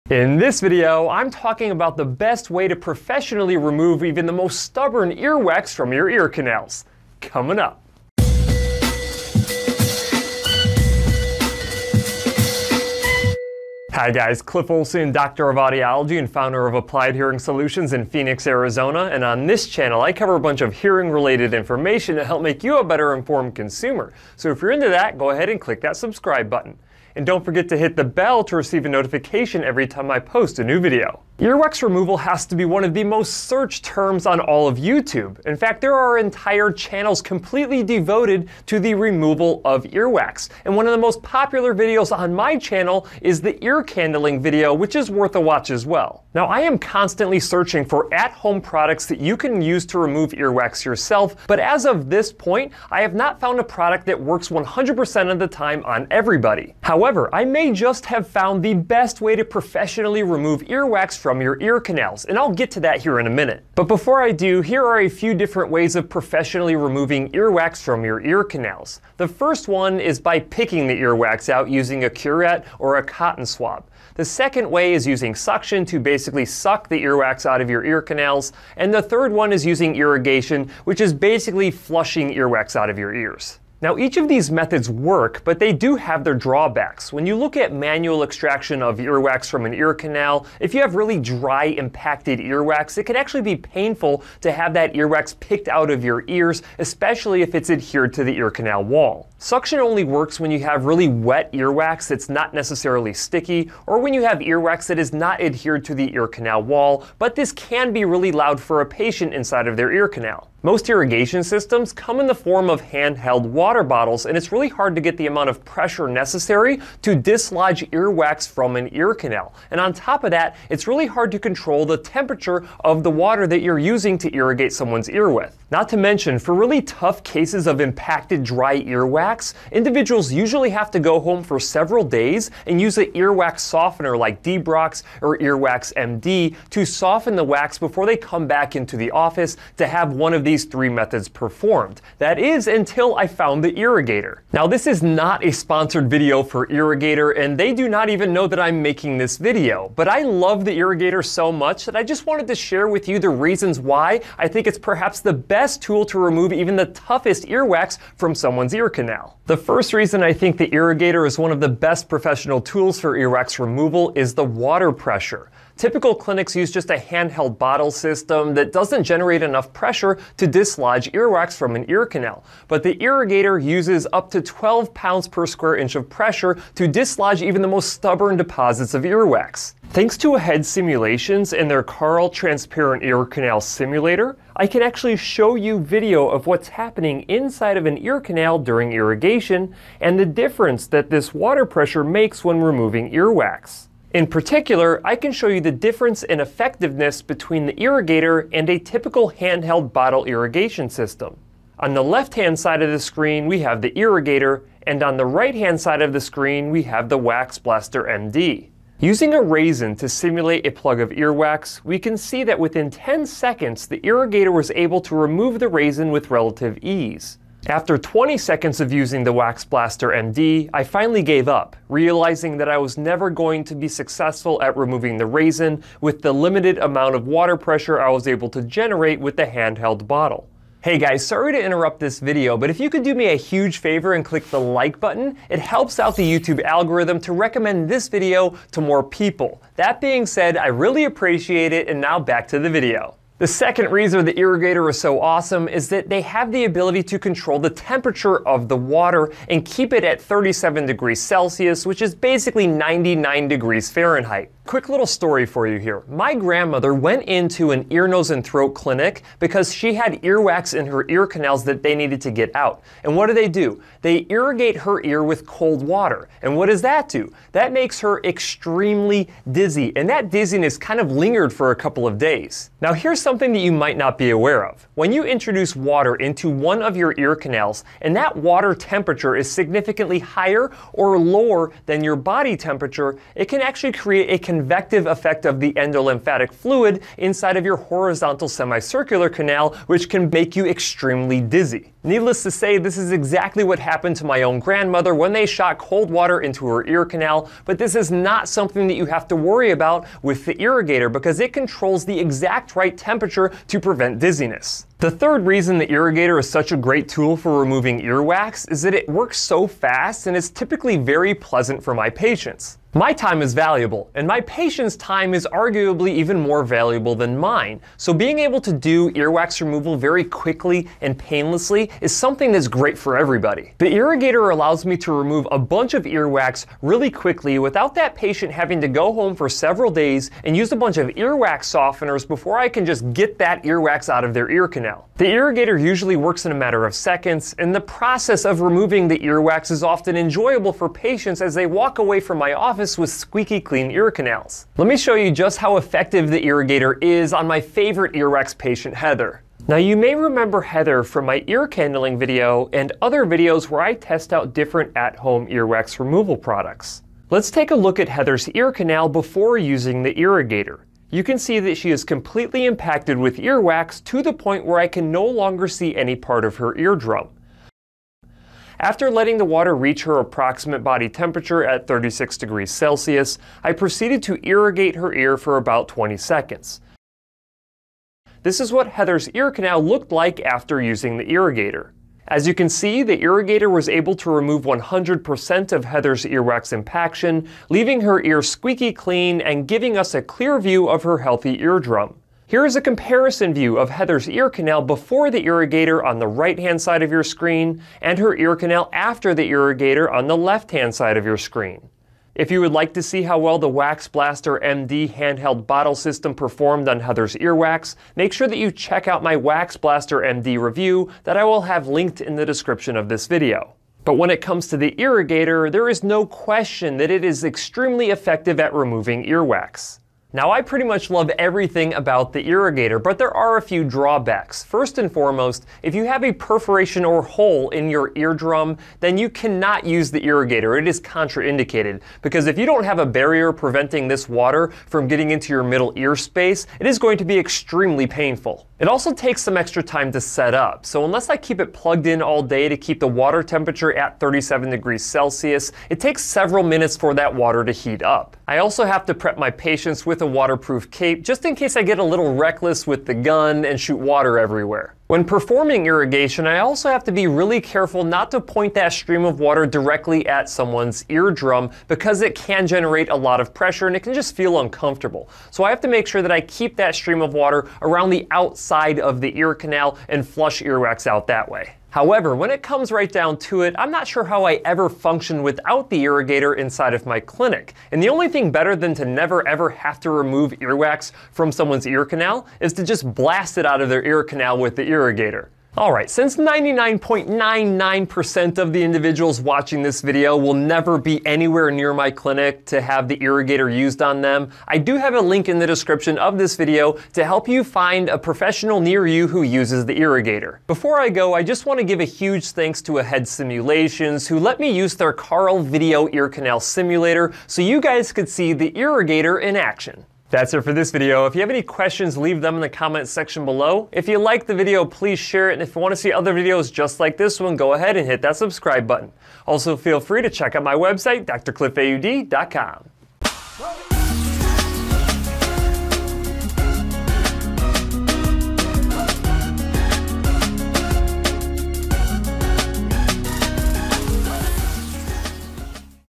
The BEST Way to Professionally Remove Earwax | Earigator Cerumen Irrigation
Professional Ear Wax Removal | Earigator Cerumen Irrigation Review.  Dr. Cliff Olson, Audiologist and founder of Applied Hearing Solutions in Phoenix Arizona, discusses perhaps the Best way to professionally irrigate earwax from your ear canal using the Earigator.  Better than at home earwax removal!